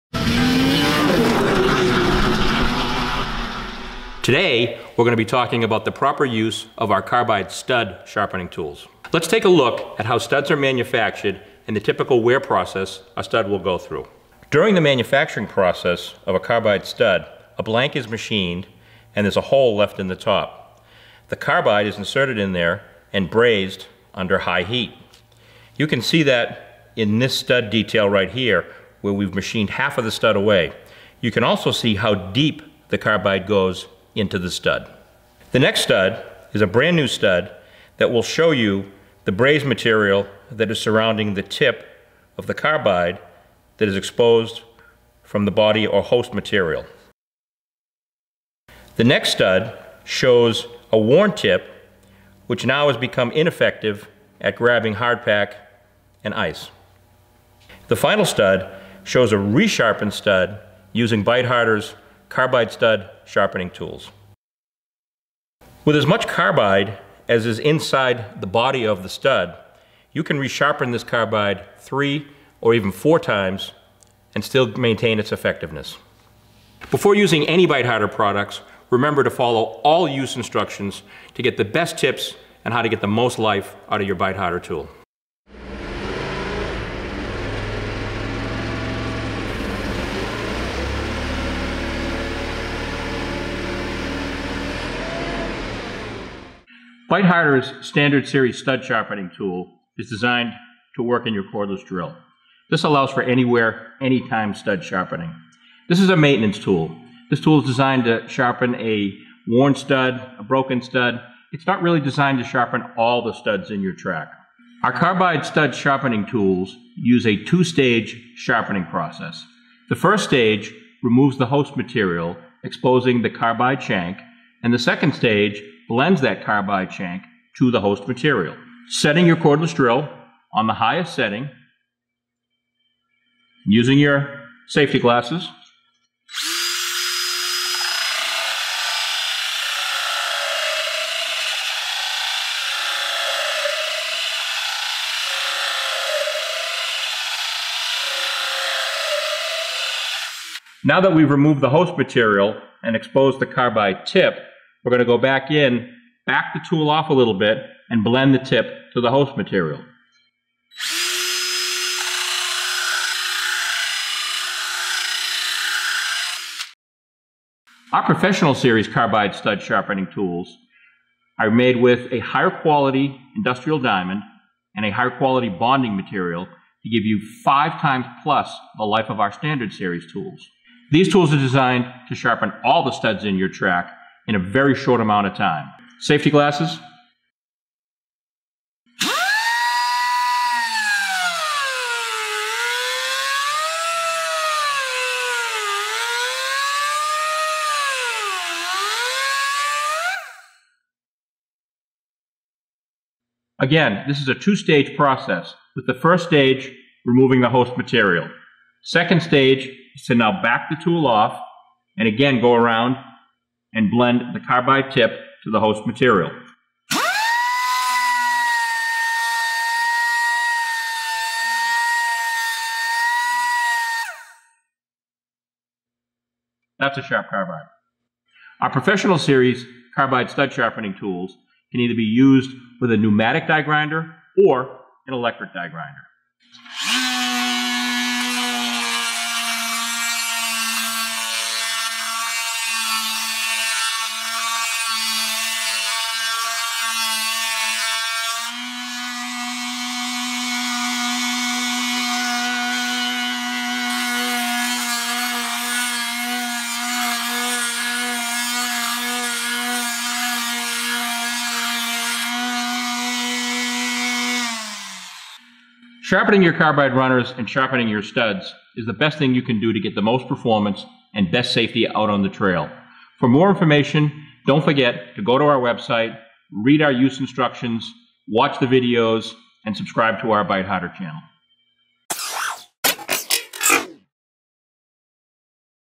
BITEHARDER CHANNEL (1/25/19) - Carbide Stud Sharpening Tools
BITEHARDER’s line of STD and PRO Series Carbide Stud Sharpening tools are detailed, including how many times a carbide stud can be sharpened and the significant cost savings that can be achieved with respect to sharpening vs. replacing.